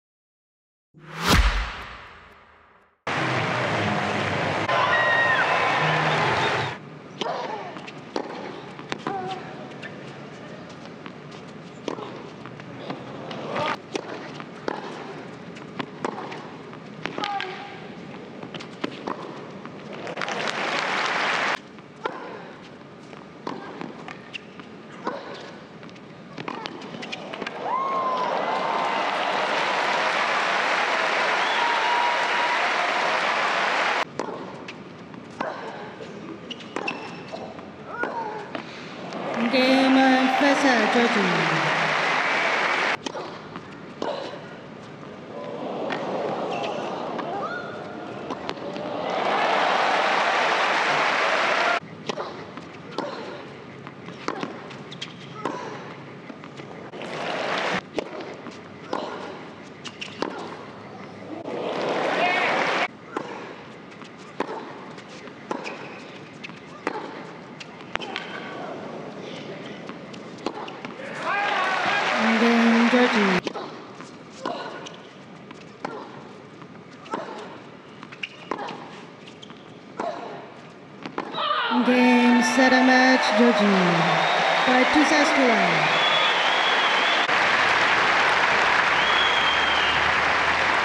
Camila Giorgi v Agnieszka Radwańska Match Highlights (QF) | Sydney International 2018
Highlights from the quarter final match between Camila Giorgi and Agnieszka Radwańska.